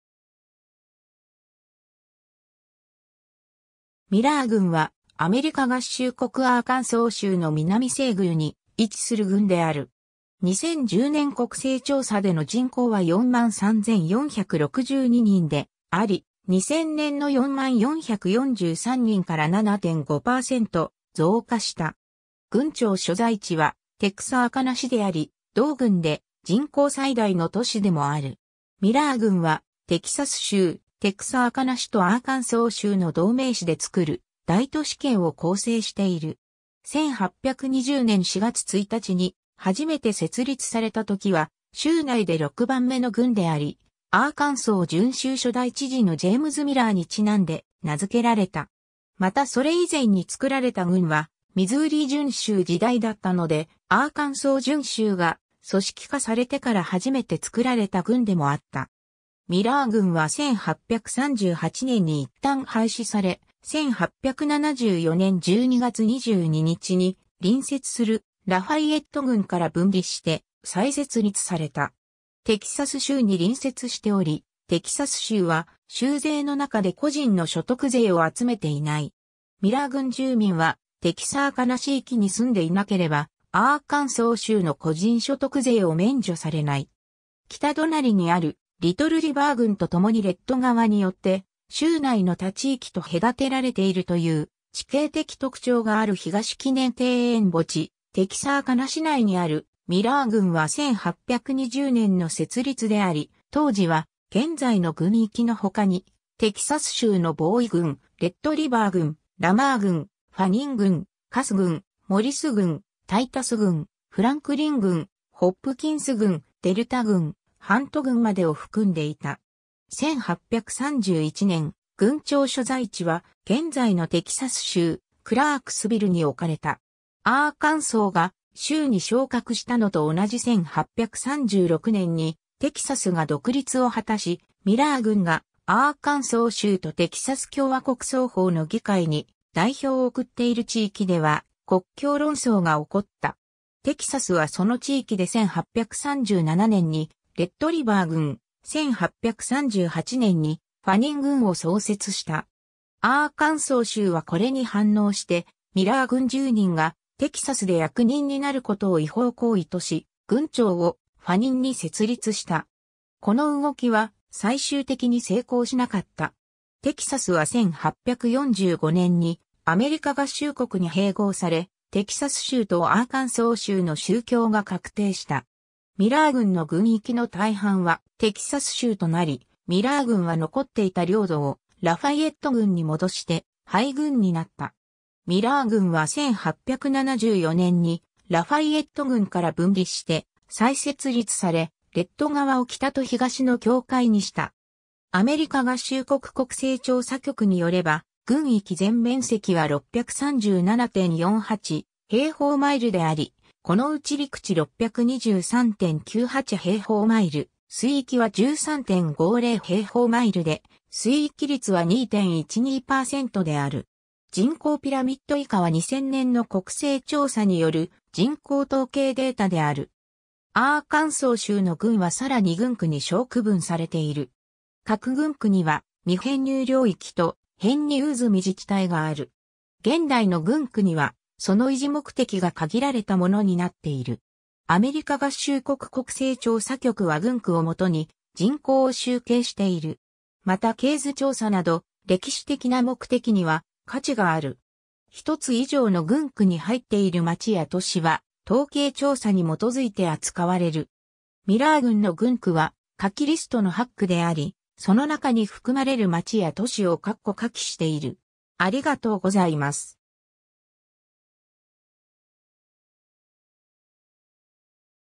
ミラー郡 (アーカンソー州)
ミラー郡（英: Miller County）は、アメリカ合衆国アーカンソー州の南西隅に位置する郡である。
2010年国勢調査での人口は43,462人であり、2000年の40,443人から7.5%増加した。
郡庁所在地はテクサーカナ市（人口29,919人）であり、同郡で人口最大の都市でもある。
ミラー郡はテキサス州テクサーカナ市とアーカンソー州の同名市（事実上は州境に跨る1つの都市）で作る大都市圏を構成している。
1820年4月1日に初めて設立されたときは州内で6番目の郡であり、アーカンソー準州初代知事のジェイムズ・ミラーに因んで名付けられた。
またそれ以前に作られた郡はミズーリ準州時代だったので、アーカンソー準州が組織化されてから初めて作られた郡でもあった。
 ミラー郡は1838年に一旦廃止され、1874年12月22日に隣接するラファイエット郡から分離して再設立された。
 テキサス州に隣接しており、テキサス州は州税の中で個人の所得税を集めていない。
ミラー郡住民はテキサーカナ市域に住んでいなければアーカンソー州の個人所得税を免除されない。
北隣にあるリトルリバー郡と共にレッド川によって、州内の他地域と隔てられているという地形的特徴がある 東記念庭園墓地、テキサーカナ市内にある ミラー郡は1820年の設立であり、当時は現在の郡域の他に、テキサス州のボウイ郡、レッドリバー郡、ラマー郡、ファニン郡、カス郡、モリス郡、タイタス郡、フランクリン郡、ホプキンス郡、デルタ郡、ハント郡までを含んでいた。
1831年、郡庁所在地は現在のテキサス州クラークスビルに置かれた。
アーカンソーが州に昇格したのと同じ1836年にテキサスが独立を果たし、ミラー郡がアーカンソー州とテキサス共和国双方の議会に代表を送っている地域では国境論争が起こった。
テキサスはその地域で1837年にレッドリバー郡、1838年にファニン郡を創設した。
アーカンソー州はこれに反応してミラー郡住人がテキサスで役人になることを違法行為とし、郡庁をファニンに設立した。
この動きは最終的に成功しなかった。
テキサスは1845年にアメリカ合衆国に併合され、テキサス州とアーカンソー州の州境が確定した。
ミラー郡の郡域の大半はテキサス州となり、ミラー郡は残っていた領土をラファイエット郡に戻して廃郡になった。
 ミラー郡は1874年にラファイエット郡から分離して再設立され、レッド川を北と東の境界にした。
 アーカンソー州の郡はさらに郡区に小区分されている。
各郡区には未編入領域と、編入済み自治体がある。
現代の郡区にはその維持目的が限られたものになっている。
アメリカ合衆国国勢調査局は郡区を元に人口を集計している。
また系図調査など歴史的な目的には価値がある。
1つ以上の郡区に入っている町や都市は統計調査に基づいて扱われる。
ミラー郡の郡区は下記リストの8区であり、その中に含まれる町や都市を括弧書きしている。